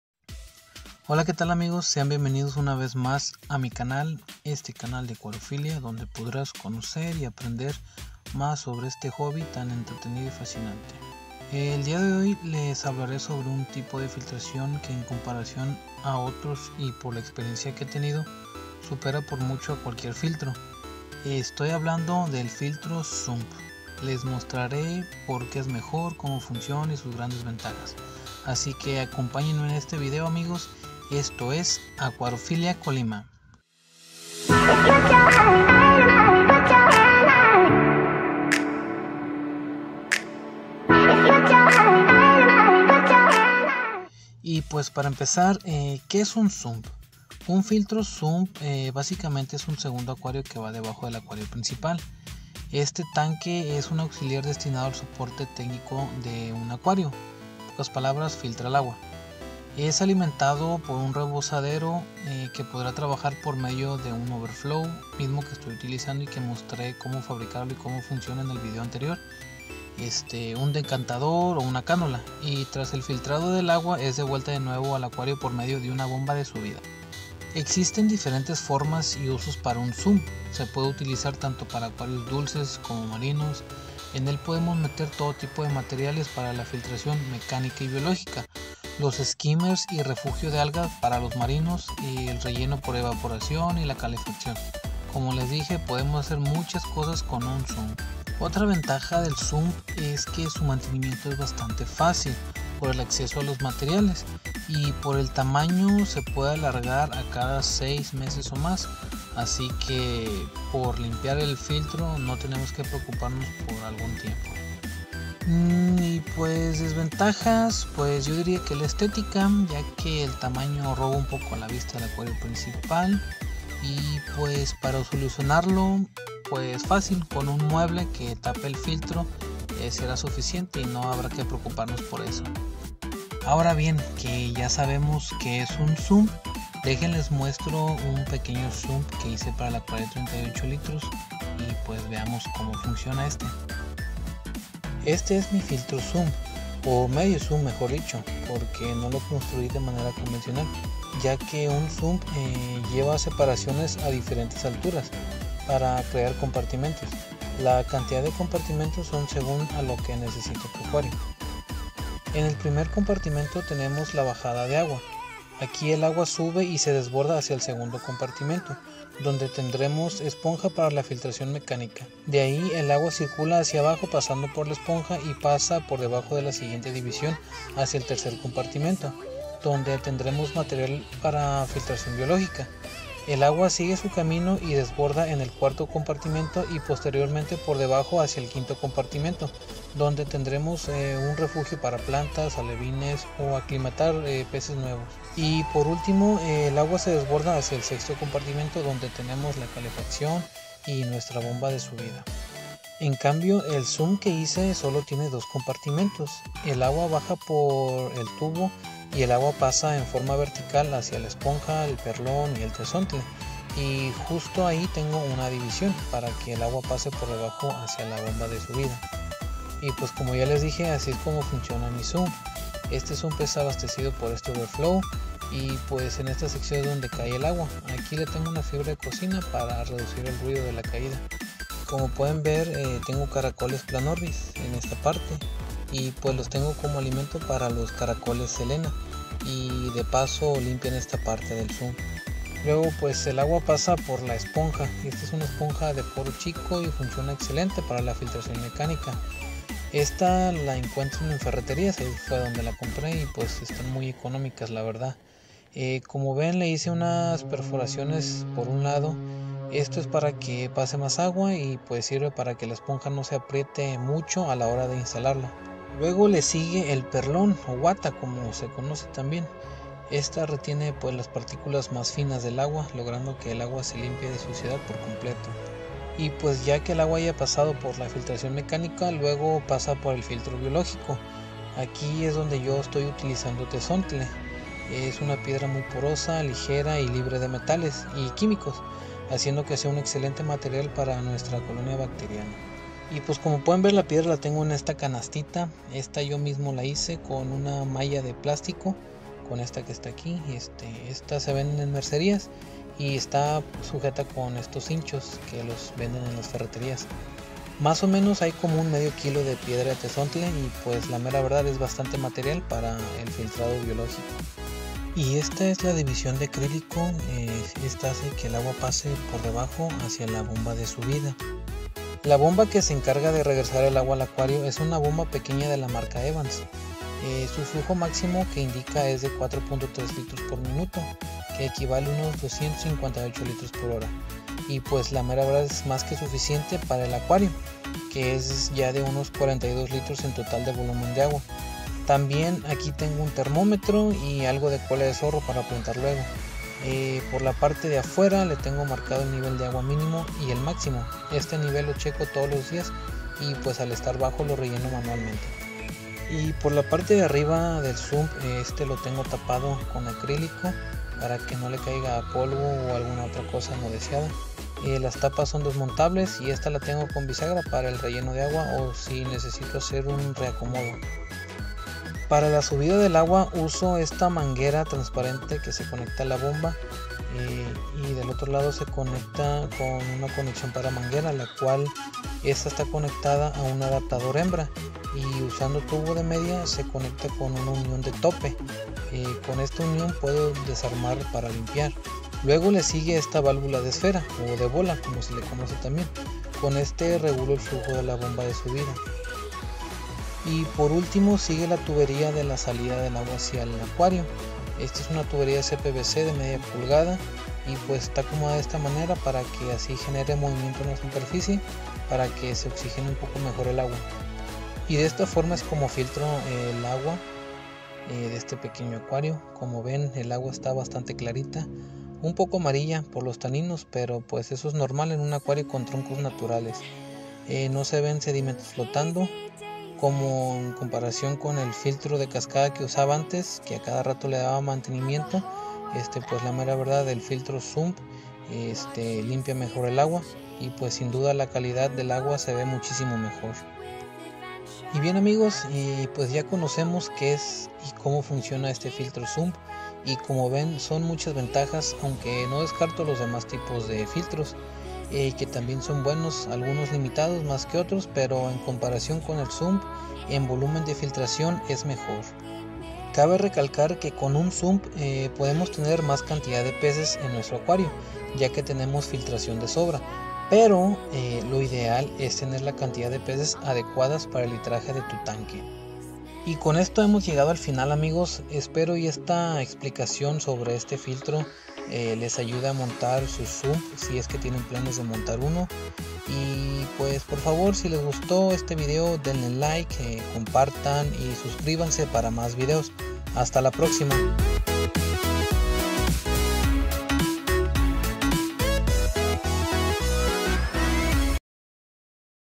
filtro SUMP👀😁, que es y COMO FUNCIONA, poniendo a PRUEBA esta FILTRACIÓN 😌🐟🌿🐚
Explicación sobre este filtro SUMP, una pequeña demostración de su función. Uno de los mejores filtros a la hora de mantenimiento y cálida del agua.
Apoya a este canal con tu suscripción y hagamos de este canal una comunidad donde podremos aprender más y más sobre este fascinante mundo de la Acuarofilia. 😁🐟🌿🐚